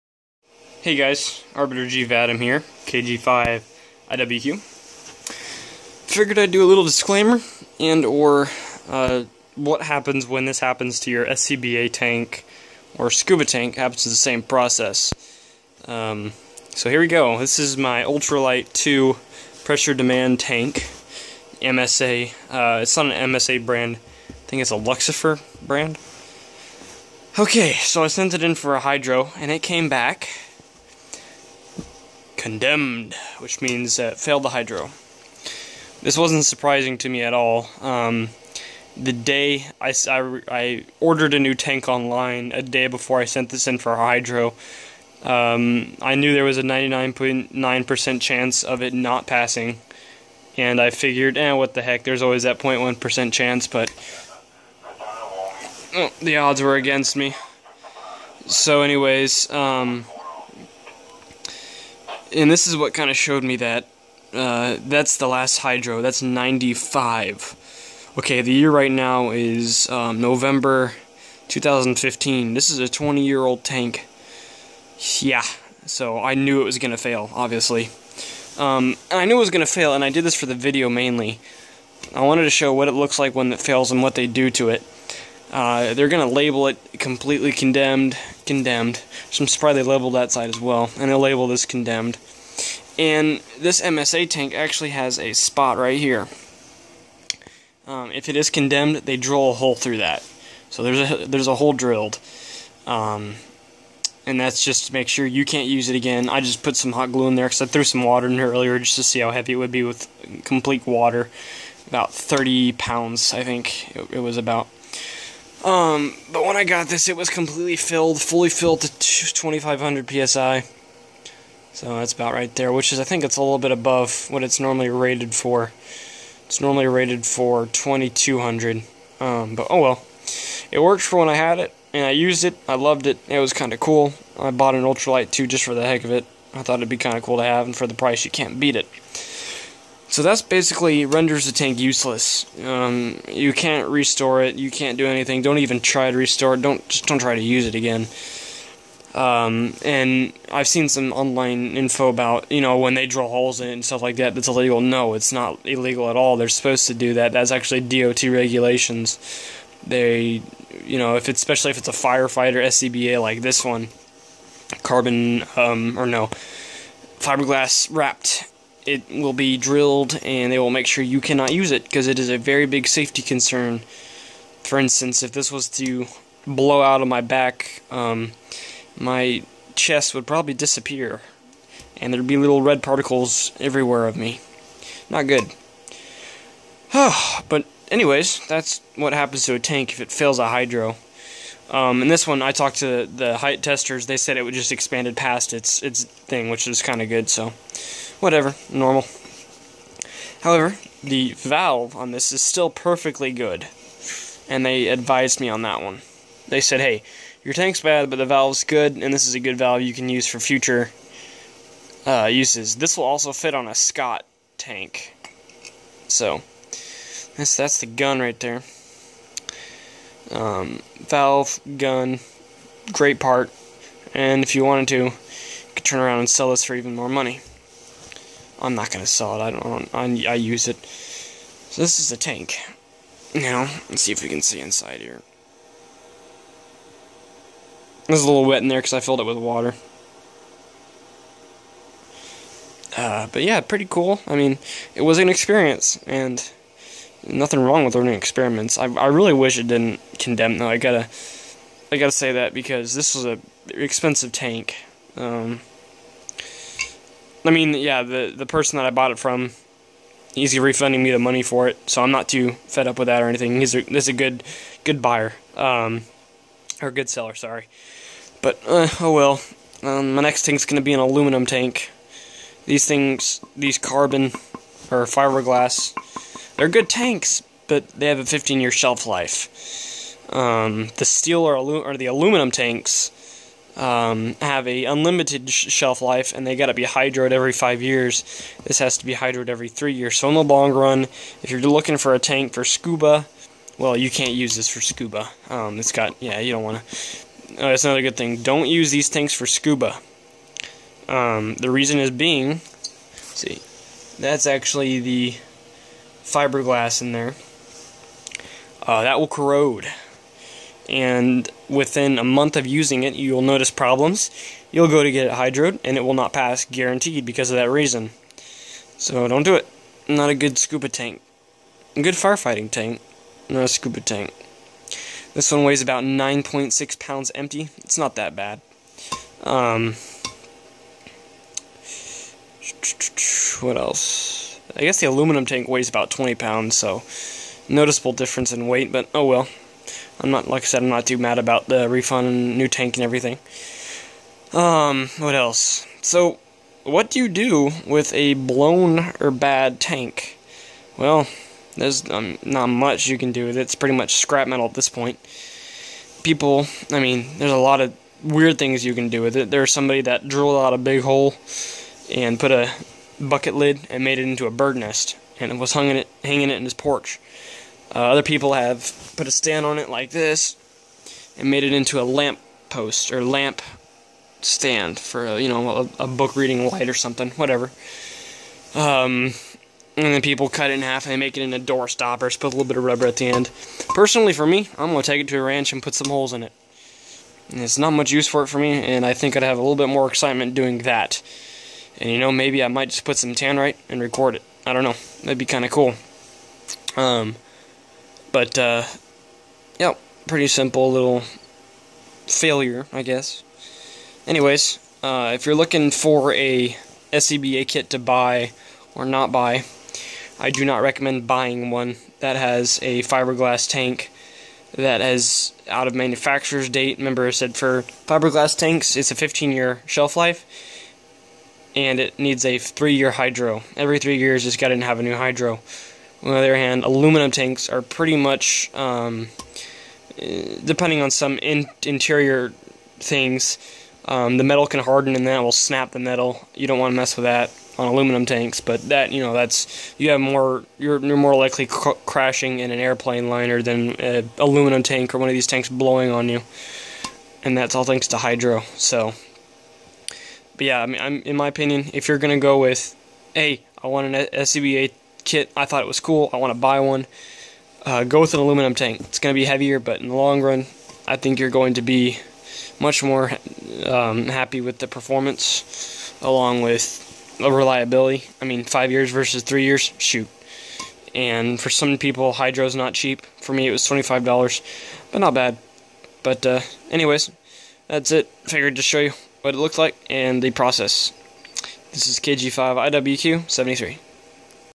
Failed hydro test
In this video I show what happens to a scba tank that has failed a hydro test.